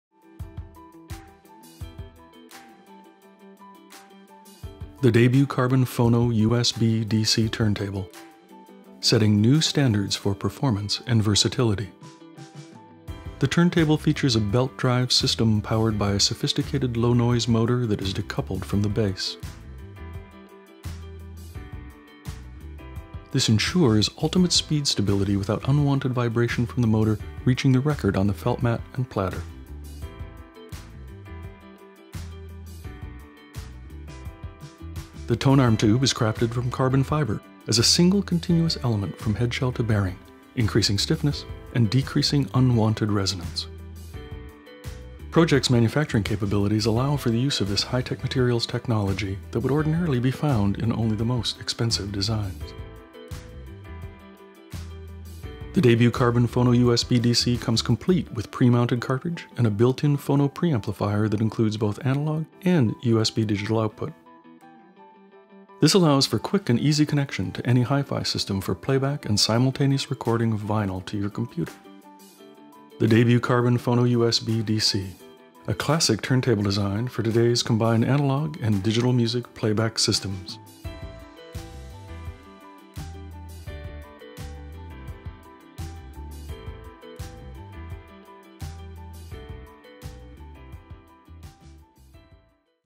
Pro-Ject Debut Carbon DC USB version
An overview of the USB and analog audio output equipped Debut Carbon DC, the benchmark against which all other USB turntables are judged.